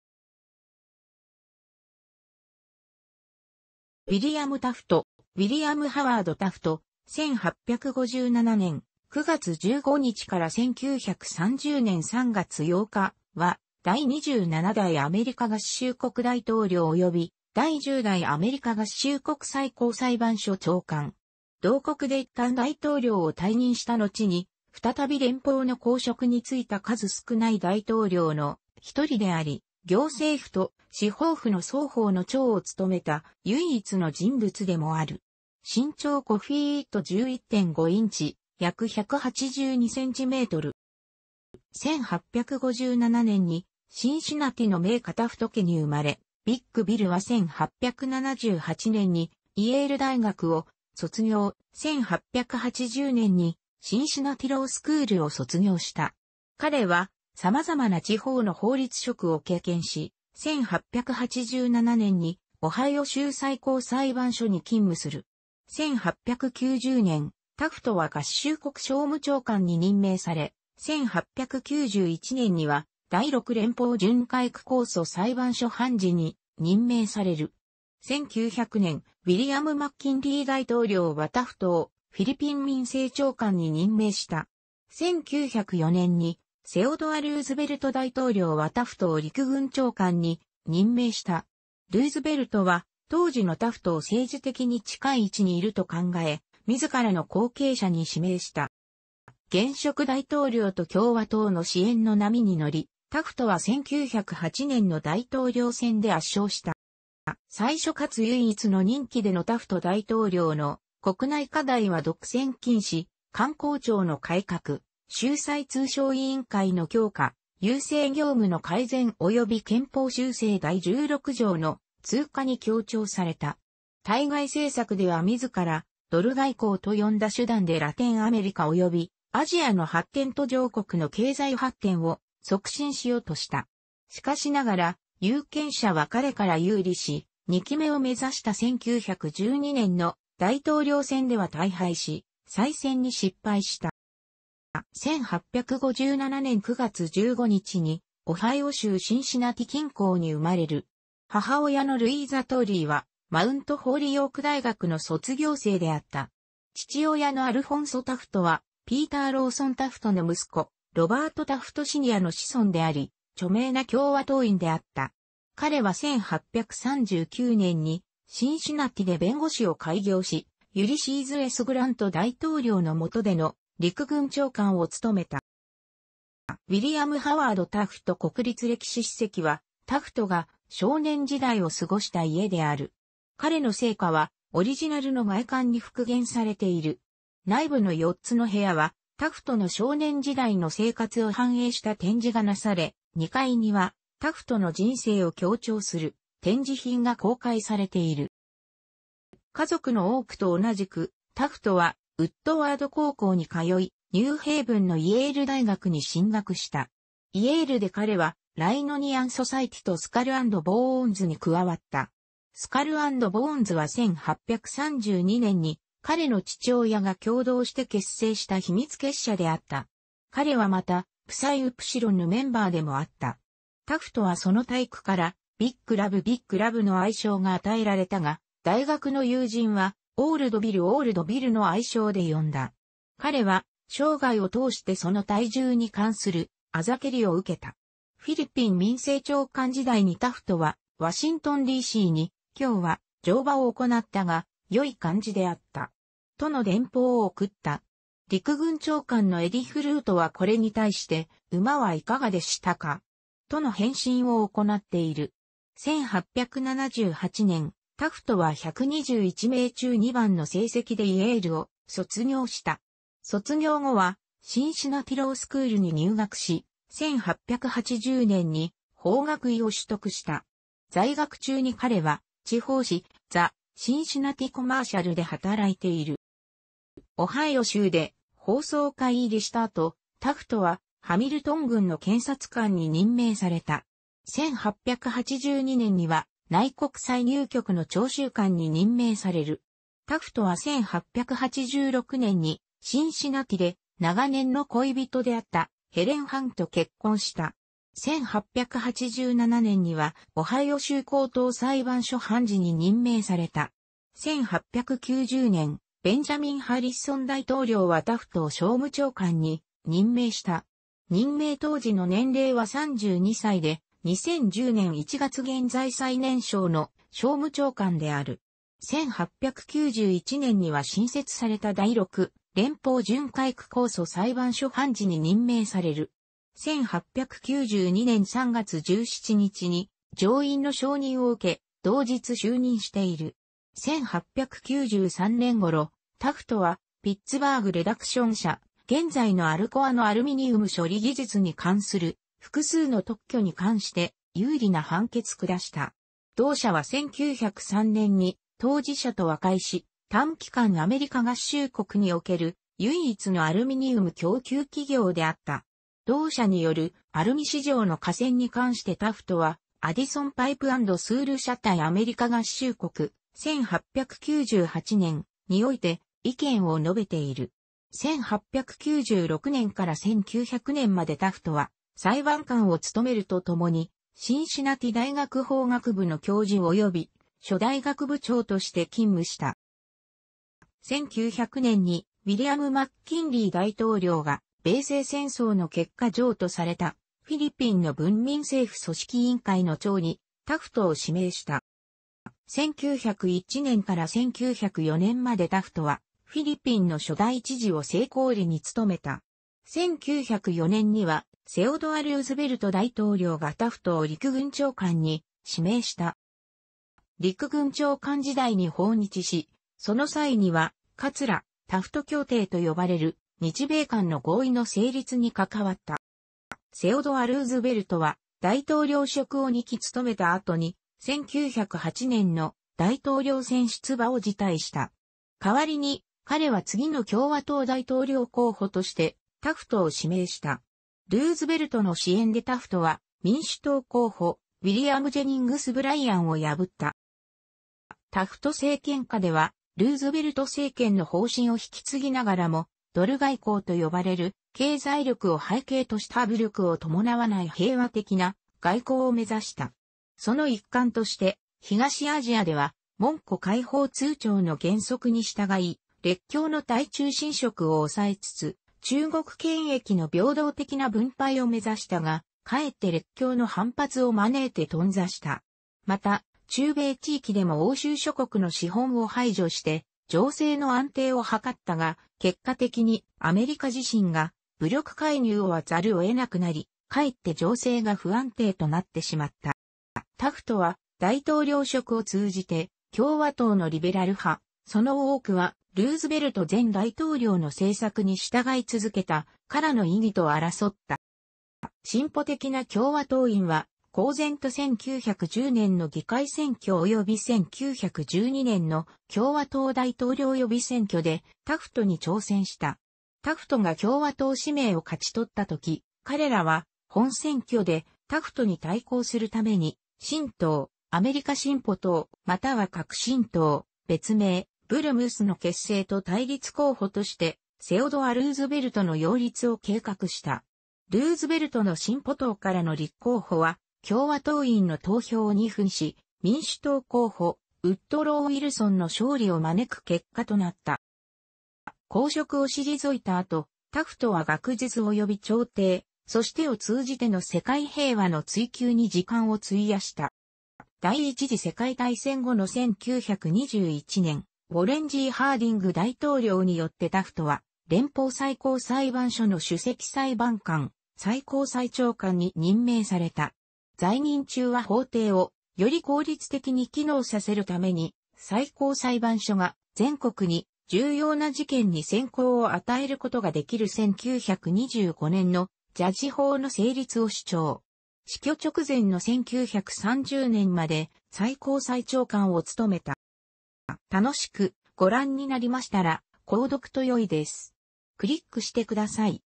ウィリアム・タフト
ウィリアム・ハワード・タフト（, 1857年9月15日 - 1930年3月8日）は、第27代アメリカ合衆国大統領および第10代アメリカ合衆国最高裁判所長官。同国でいったん大統領を退任したのちに再び連邦の公職に就いた数少ない大統領のひとりであり、行政府と司法府の双方の長を務めた唯一の人物でもある。身長5フィート11.5インチ（約182cm）。

1857年にシンシナティの名家タフト家に生まれ、「ビッグ・ビル」は1878年にイェール大学を卒業、1880年にシンシナティ・ロー・スクールを卒業した。彼は様々な地方の法律職を経験し、1887年にオハイオ州最高裁判所に勤務する。1890年、タフトは合衆国訟務長官に任命され、1891年には第6連邦巡回区控訴裁判所判事に任命される。1900年、ウィリアム・マッキンリー大統領はタフトをフィリピン民政長官に任命した。1904年にセオドア・ルーズベルト大統領はタフトを陸軍長官に任命した。ルーズベルトは当時のタフトを政治的に近い位置にいると考え、自らの後継者に指名した。

現職大統領と共和党の支援の波に乗り、タフトは1908年の大統領選で圧勝した。

最初かつ唯一の任期でのタフト大統領の国内課題は独占禁止、官公庁の改革、州際通商委員会の強化、郵政業務の改善および憲法修正第16条の通過に強調された。対外政策では自ら「ドル外交」と呼んだ手段でラテンアメリカおよびアジアの発展途上国の経済発展を促進しようとした。しかしながら、有権者は彼から遊離し、二期目を目指した1912年の大統領選では大敗し再選に失敗した。

1857年9月15日にオハイオ州シンシナティ近郊に生まれる。母親のルイーザ・トーリーはマウント・ホリヨーク大学の卒業生であった。父親のアルフォンソ・タフトはピーター・ローソン・タフトの息子、ロバート・タフト・シニアの子孫であり、著名な共和党員であった。彼は1839年にシンシナティで弁護士を開業し、ユリシーズ・S・グラント大統領の元での陸軍長官を務めた。

ウィリアム・ハワード・タフト国立歴史史跡は、タフトが少年時代を過ごした家である。彼の生家はオリジナルの外観に復元されている。内部の4つの部屋は、タフトの少年時代の生活を反映した展示がなされ、2階にはタフトの人生を強調する展示品が公開されている。

家族の多くと同じく、タフトはウッドワード高校に通い、ニューヘイヴンのイェール大学に進学した。イェールで彼はライノニアン・ソサエティとスカル・アンド・ボーンズに加わった。スカル・アンド・ボーンズは1832年に彼の父親が共同して結成した秘密結社であった。彼はまた、プサイ・ウプシロンのメンバーでもあった。タフトはその体躯から「ビッグ・ラブ "Big Lub"」の愛称が与えられたが、大学の友人は「オールド・ビル "Old Bill"」の愛称で呼んだ。彼は生涯を通してその体重に関する嘲りを受けた。フィリピン民政長官時代にタフトはワシントンD.C.に「今日は乗馬を行ったが、良い感じであった。」との電報を送った。陸軍長官のエリフ・ルートはこれに対して「馬はいかがでしたか？」との返信を行っている。1878年、タフトは121名中2番の成績でイェールを卒業した。卒業後はシンシナティ・ロー・スクールに入学し、1880年に法学位を取得した。在学中に彼は地方紙「"The Cincinnati Commercial"」で働いている。

オハイオ州で法曹界入りした後、タフトはハミルトン郡の検察官に任命された。1882年には内国歳入局の徴収官に任命される。タフトは1886年にシンシナティで長年の恋人であったヘレン・ハロンと結婚した。1887年にはオハイオ州高等裁判所判事に任命された。1890年、ベンジャミン・ハリソン大統領はタフトを訟務長官に任命した。任命当時の年齢は32歳で、2010年1月現在最年少の訟務長官である。1891年には新設された第6連邦巡回区控訴裁判所判事に任命される。1892年3月17日に上院の承認を受け、同日就任している。1893年頃、タフトはピッツバーグ・レダクション社（現在のアルコア）のアルミニウム処理技術に関する複数の特許に関して有利な判決を下した。同社は1903年に当事...